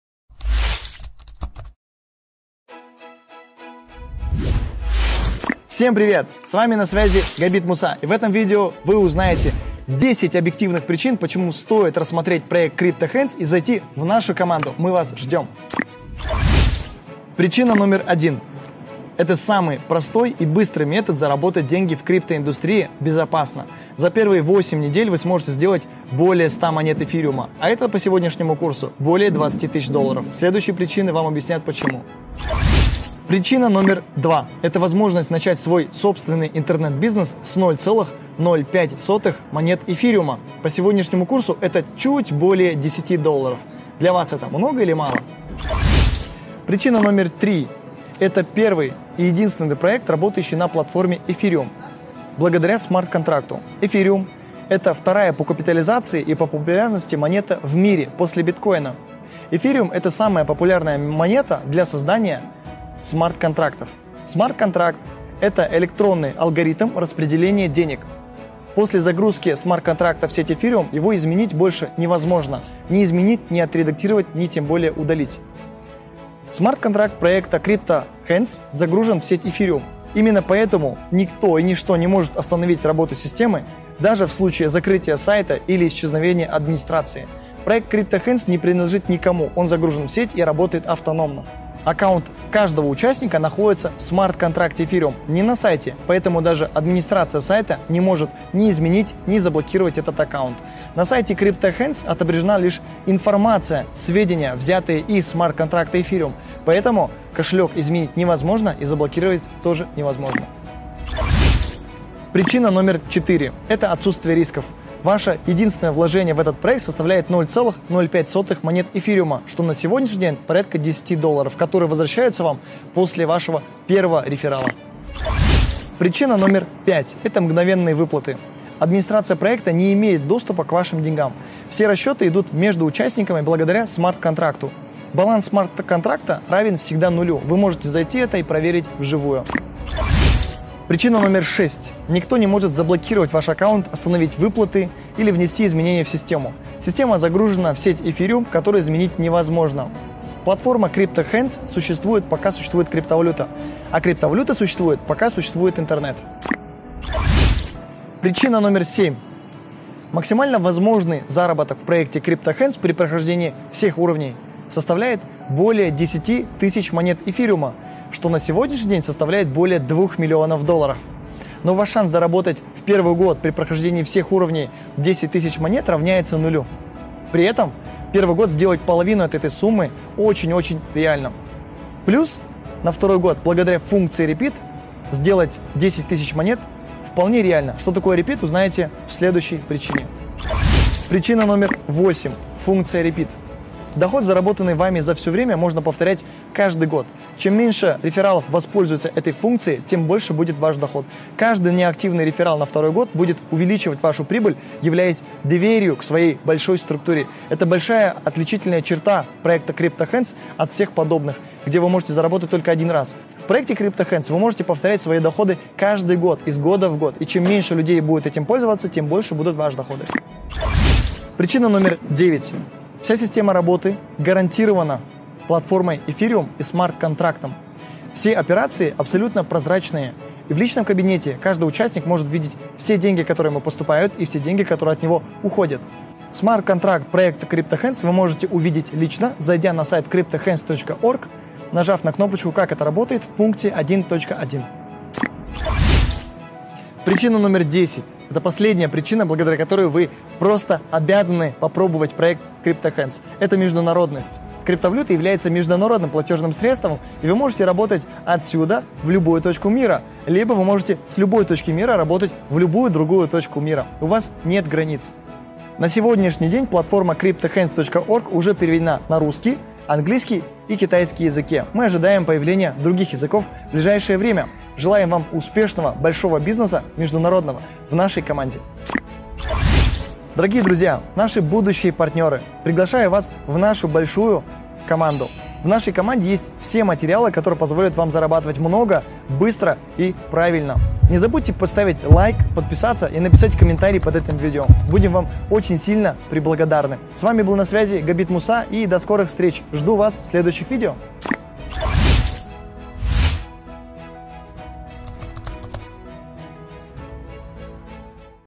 ТОП 10 ПРИЧИН ВЫБРАТЬ CRYPTOHANDS
Cryptohands - самый уникальный безрисковый проект!

!!! Здесь не нужно подтверждать переводы или заказывать выплаты - средства сразу поступают к Вам на кошелёк и не задерживаются в системе, что сводит риск потери заработанных Вами денег к нулю!

!!! Нет угрозы закрытия – аккаунт каждого участника находится в смарт-контракте (это значит, что после загрузки в сеть его больше нельзя отредактировать или изменить), а не на сайте, поэтому не может быть заблокирован или удален даже администрацией сайта!

!!! Нет никаких владельцев

!!! Проект работает по принципу краудфандинга (от человека к человеку)

Другими словами - это безрисковый проект, который исключает человеческий фактор. Наиболее хорошо подходит для быстрого и легкого интернет-заработка, как для новичков, так и для опытных сетевиков.

Для регистрации в проекте перейдите к подробной видео-инструкции и начинайте зарабатывать первые деньги уже сегодня!!!

По всем вопросам обращайтесь:
skype: pavel.osipov.egorov